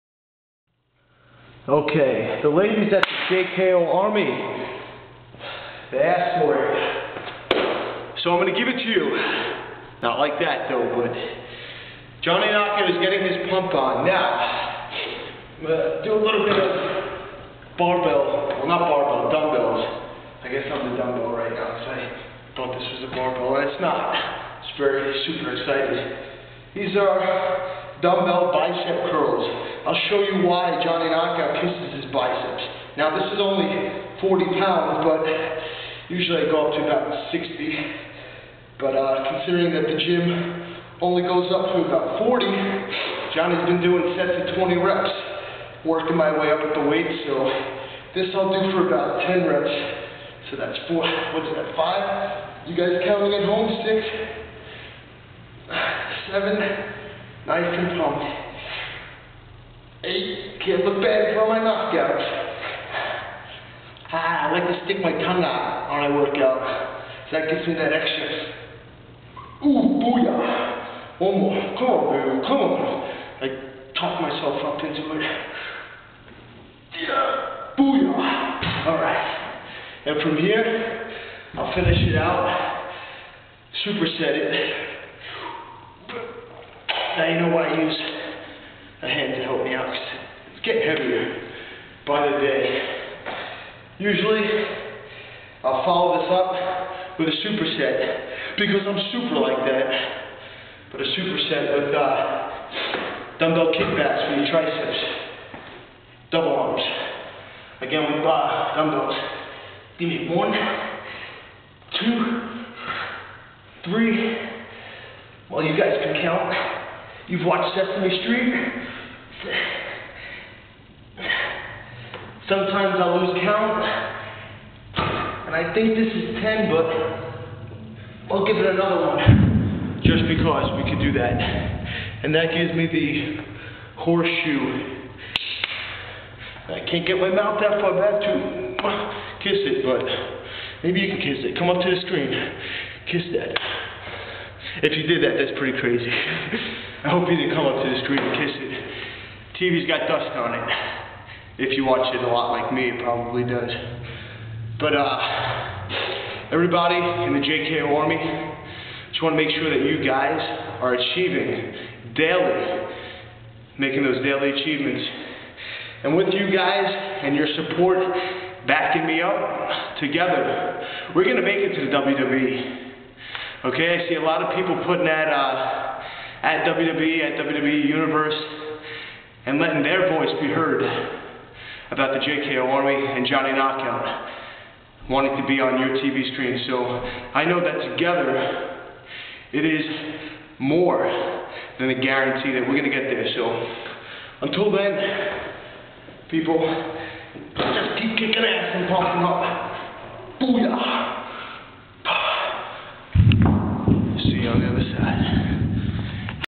JKOReality Episode#23 - KNOCK+OUT WORKOUT!
As requested by YOU! The JKOArmy, JKO gives you an 'inside look' at why he kisses his biceps in "The KNOCK+OUT WORKOUT!" BOO-Yah! ..and he thanks you for Your Support ;)